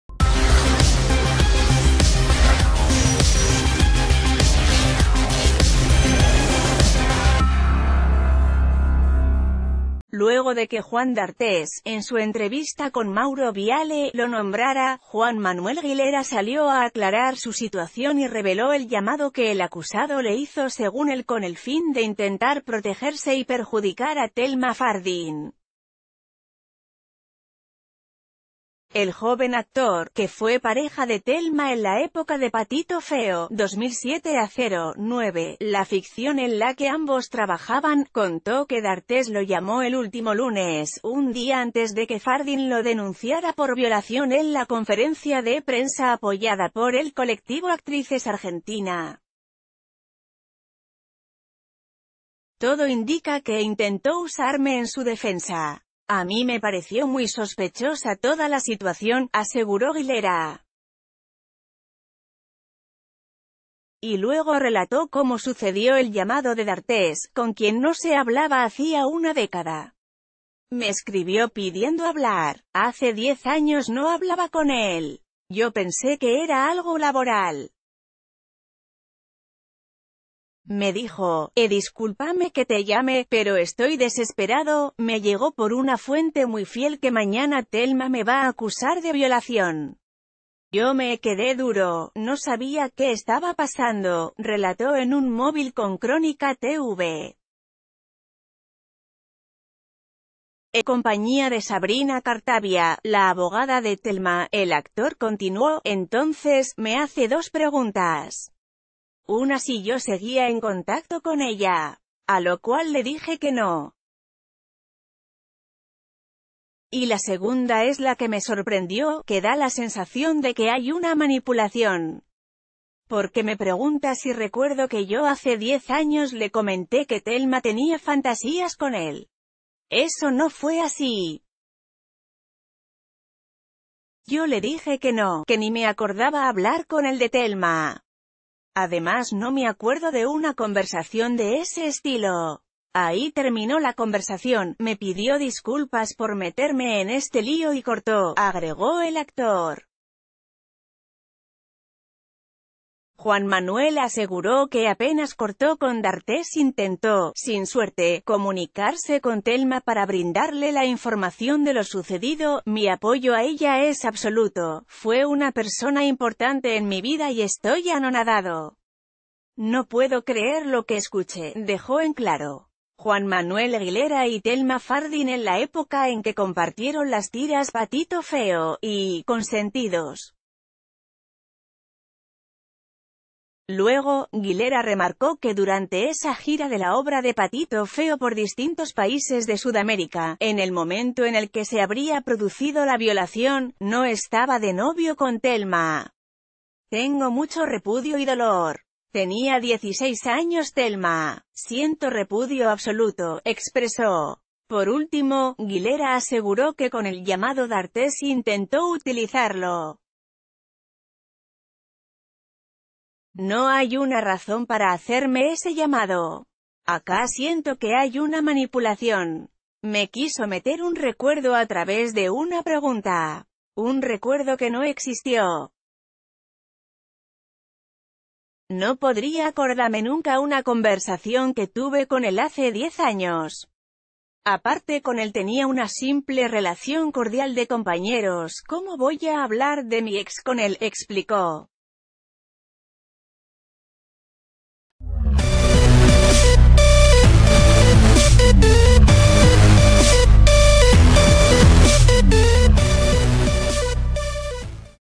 Juan Manuel Guilera: "Darthés intentó manipularme para perjudicar a Thelma Fardin"
Luego de que Juan Darthés, en su entrevista con Mauro Viale, lo nombrara, Juan Manuel Guilera salió a aclarar su situación y reveló el llamado que el acusado le hizo -según él- con el fin de intentar protegerse y perjudicar a Thelma Fardin. El joven actor, que fue pareja de Thelma en la época de Pat...